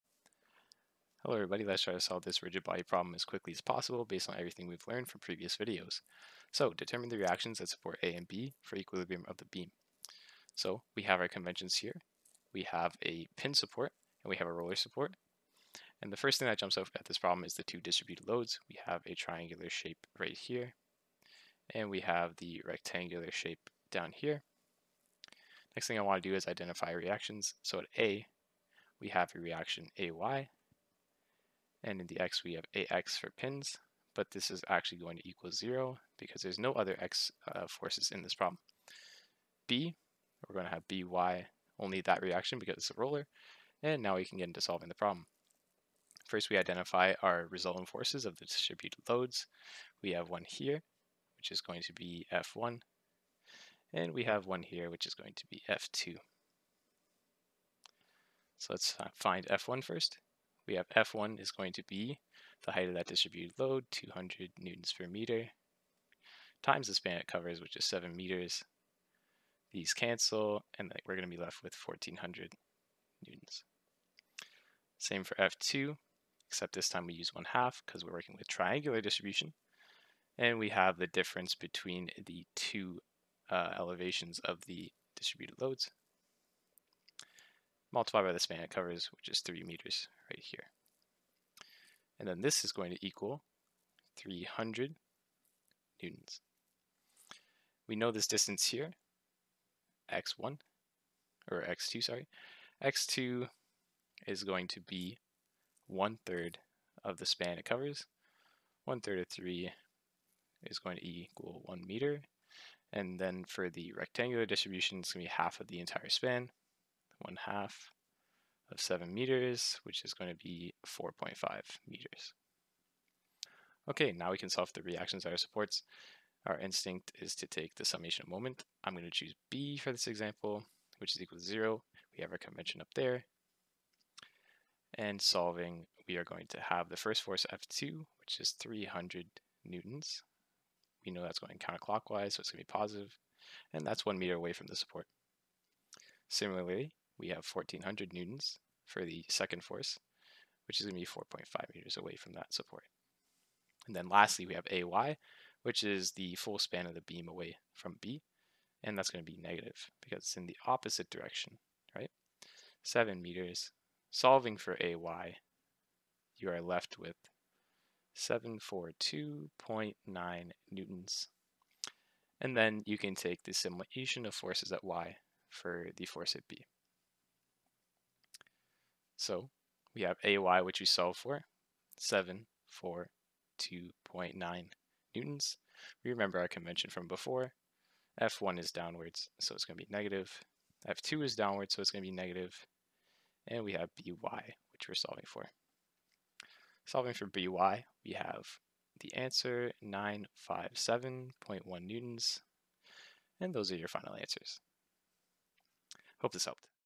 How to Solve Equilibrium of Rigid Bodies Quick!
Question *5-92: Determine the reactions at the supports A and B
for equilibrium of the beam.

Took a different approach to solving this one! Wanted to be as quick as possible while also explaining all steps. Please leave any comments regarding questions you may have, or general recommendations for future problems.